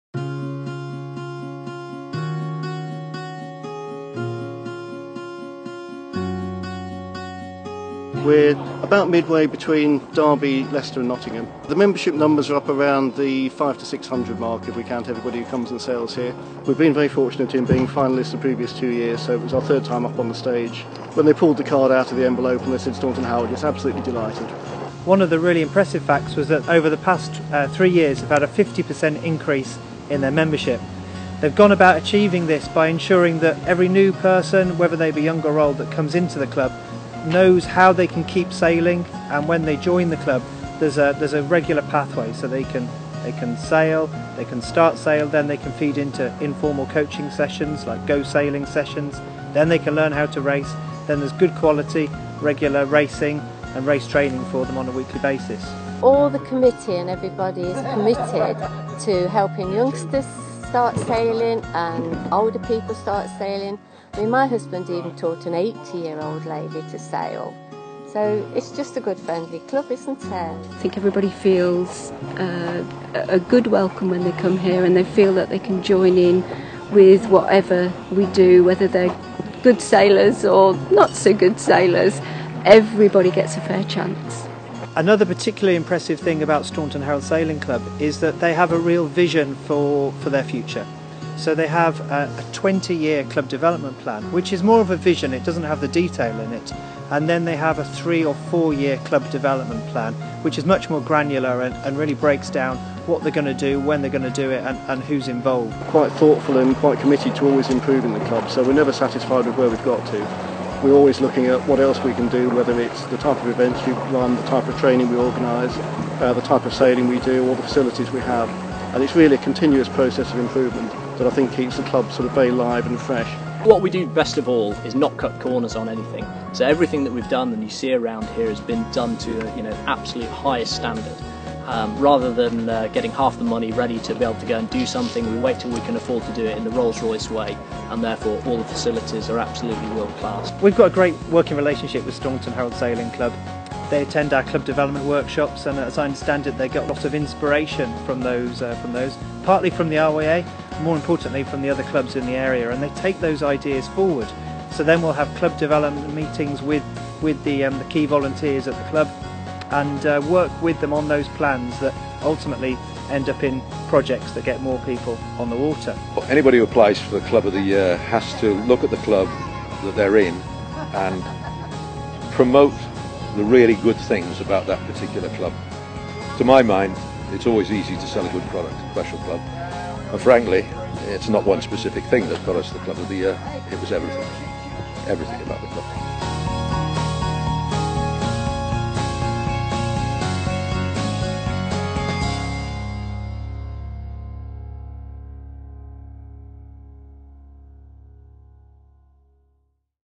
RYA Club of the Year 2014 - Staunton Harold Sailing Club
Staunton Harold Sailing Club is RYA Club of the Year 2014.

 The club, which has been a finalist three years in a row, received their award from the British Sailing Team’s Megan Pascoe and RYA CEO Sarah Treseder at the RYA Suzuki Dinghy Show on Saturday 01 March.